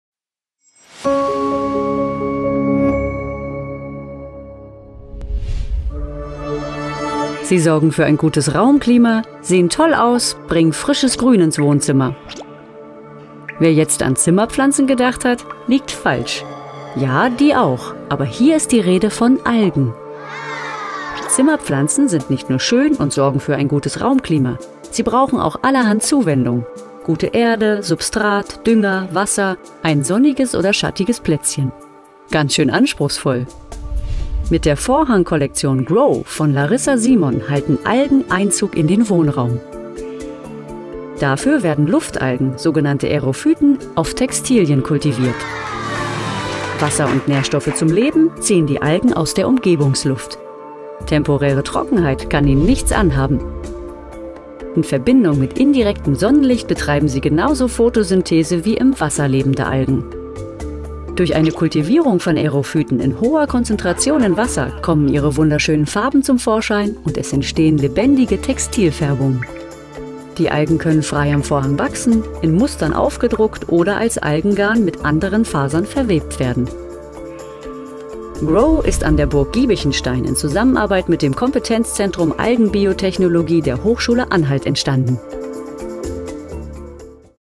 BESTFORM-Award 2019 Vision des Jahres: Collection GROW
BESTFORM 2019: Das sind die Preisträger!

Das Projekt „Collection GROW“ von Larissa Siemon aus Halle (Saale) wurde mit dem Titel „Vision des Jahres“ ausgezeichnet. Die Produktdesignerin erhielt für ihre Idee, Luftalgen als „lebende Textilfarben“ einzusetzen und damit einzigartige und umwelt- freundliche Farben zu erzeugen, ein Preisgeld von 2.500 Euro. Die Jury begründete ihre Entscheidung so: „GROW ist ein spannender Lösungsweg, der uns eine interessante Symbiose von Natur, Pflanze und Mensch zeigt – und das in einer hoch ästhetischen Qualität. Es geht um die Verbesserung der Luft, die wir zum Atmen brauchen.“